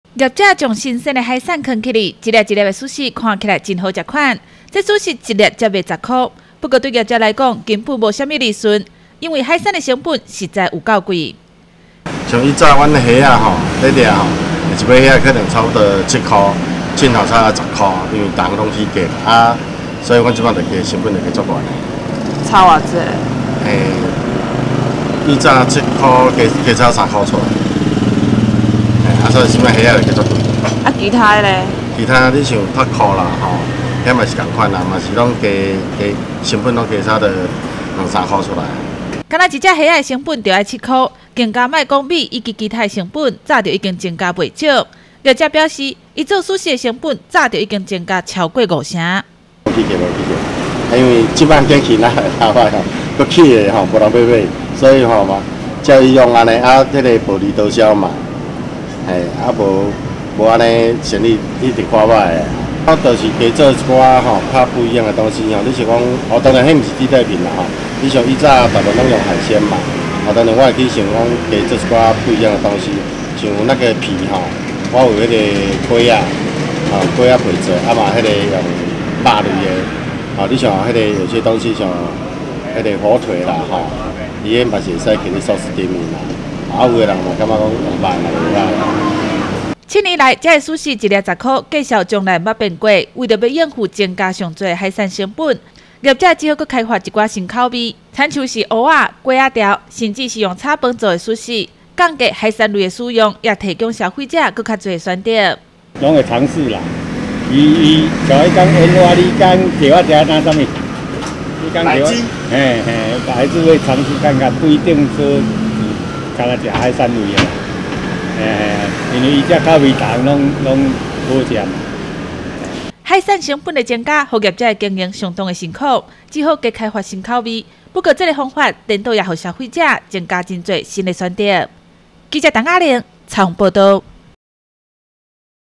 雲林新聞網-西螺十元壽司苦撐
壽司經常會用到許多海鮮，不過受到海鮮成本不斷飆漲，對於壽司業者來說，成本增加不少，西螺有一間經營七年的十元壽司業者，多年來不曾漲價，但是壽司成本早就增加超過五成，因為不敢漲價，業者只好開發更多新口味的壽司，來替代部分海鮮，降低成本負擔。